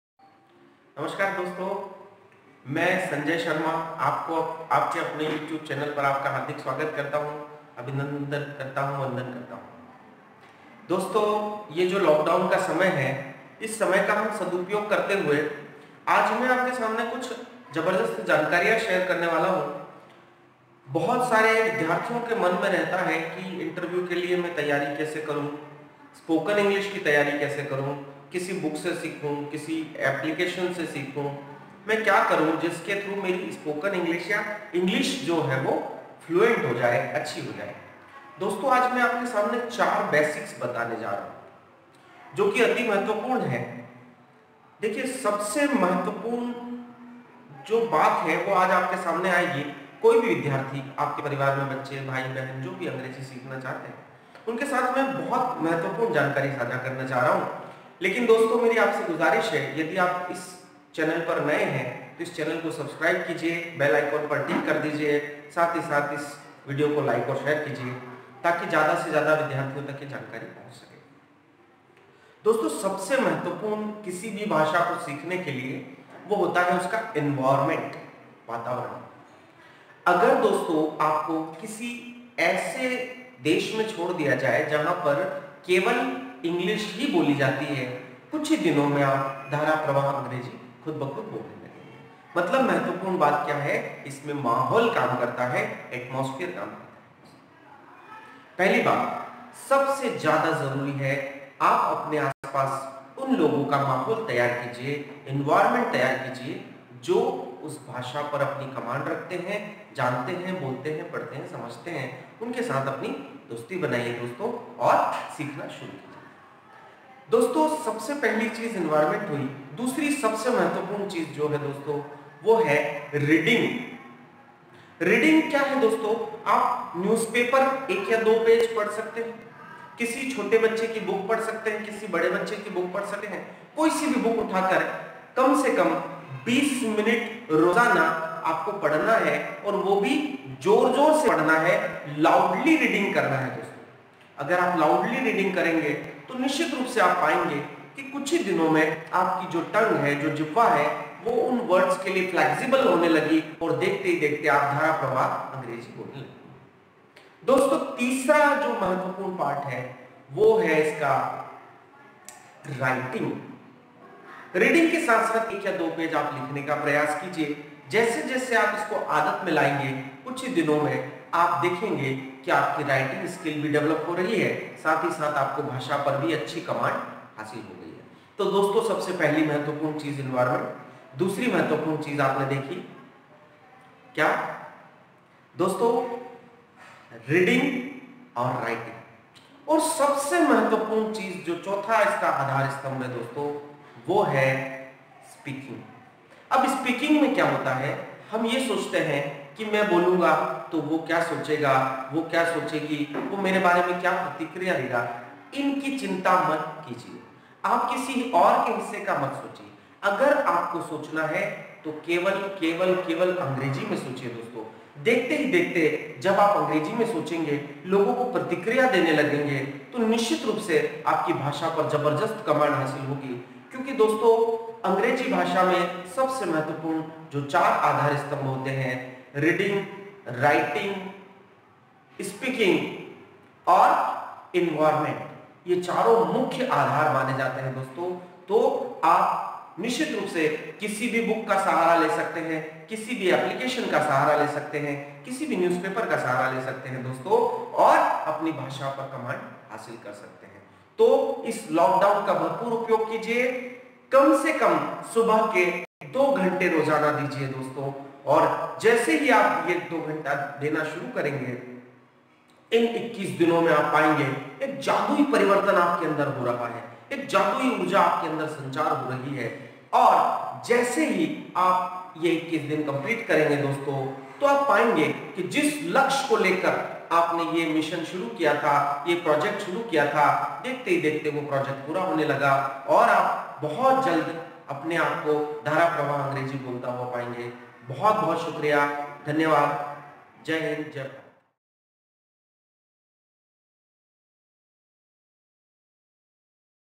4 MAIN PILLARS OF SPOKEN ENGLISH
You will have to put yourself in English speaking community.and try to speak and write and think in English .. Reading English books is compulsory..